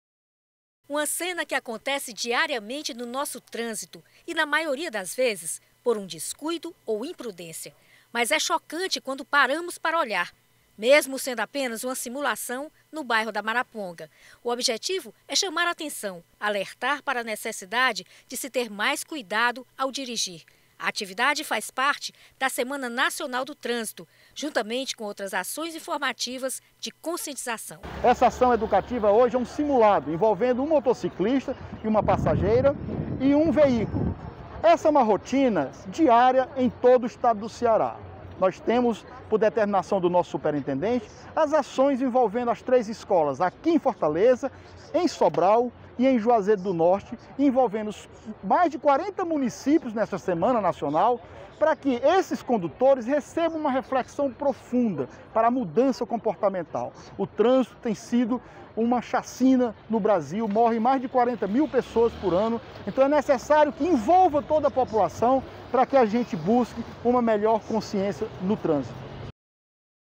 SIMULAÇÃO FAZ ALERTA SOBRE ACIDENTES NA SEMANA NACIONAL DO TRÂNSITO
JornaldaTVC | O Detran Ceará está realizando uma programação especial com atividades educativas e de conscientização na Semana Nacional do Trânsito. Em Fortaleza, de acordo com a
AMC,  noventa e oito pessoas perderam a vida em acidentes de trânsito  entre janeiro e junho de 2021.